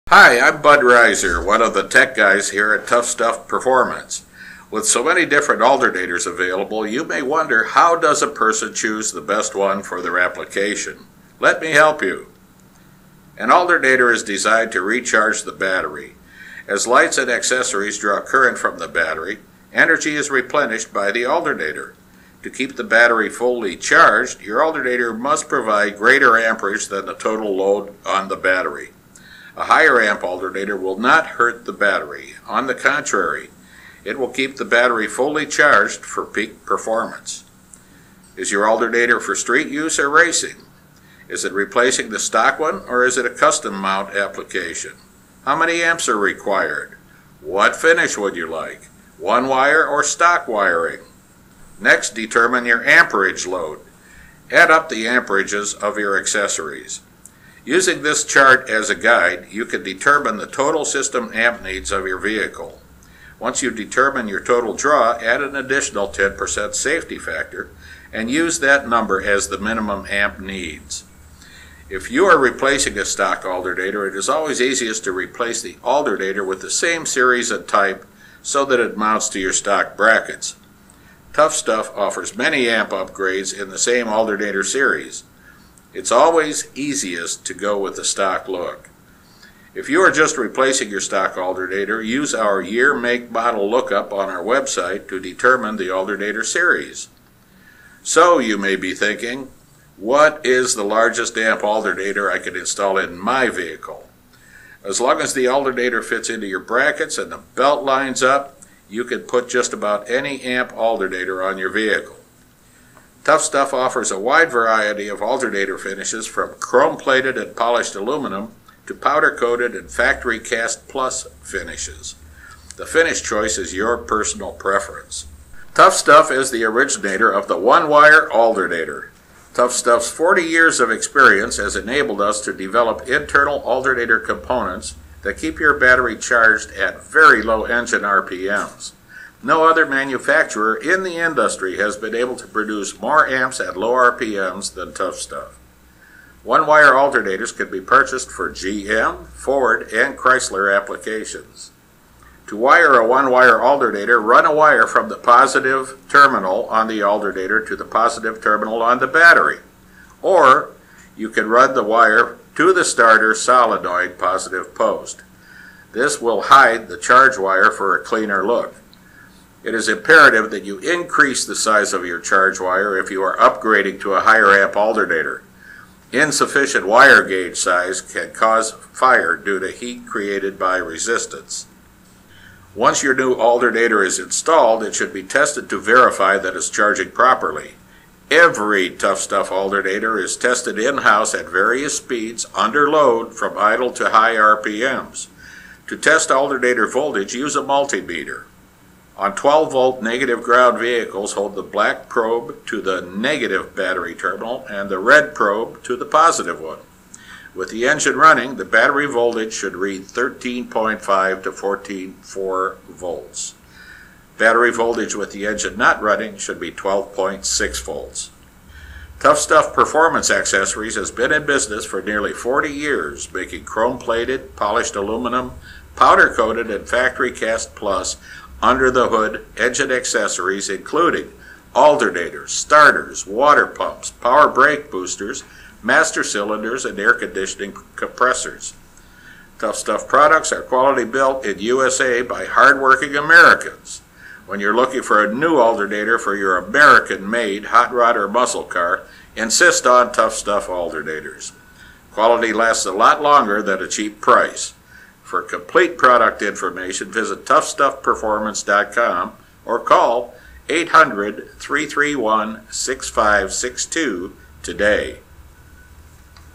Choosing an Alternator
How to determine the best alternator for your vehicle.